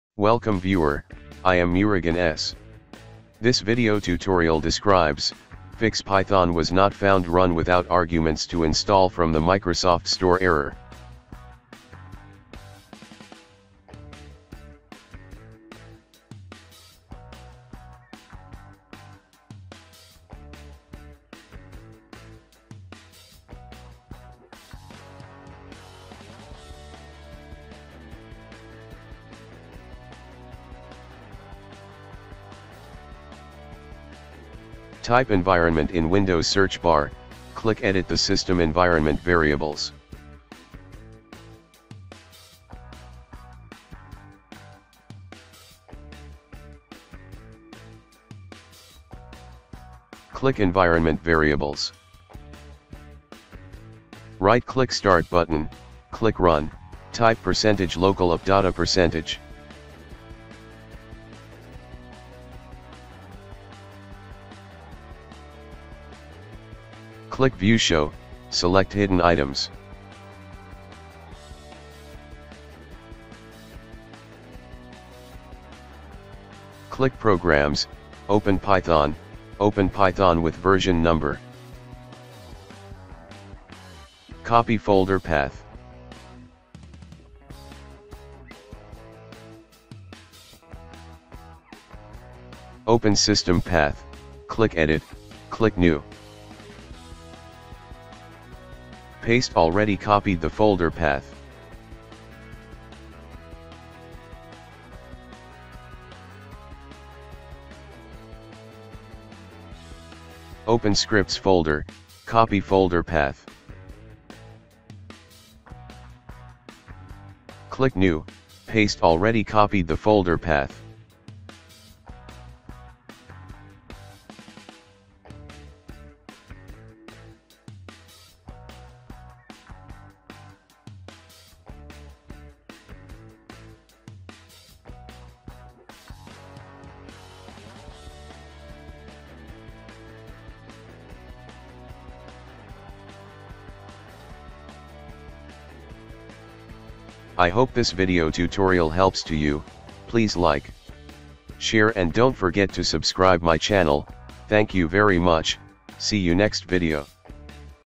Fix Python Was Not Found Run Without Arguments To Install From The Microsoft Store In Windows 11/10
This Tutorial Helps to Fix Python Was Not Found Run Without Arguments To Install From The Microsoft Store In Windows 11/10

00:00 Intro
00:10 Open Command Prompt
00:25 System Environment Variable
00:54 Open Local App Data Folder
01:11 Show Hidden Items
01:39 Add New Environment Variable
02:27 Closing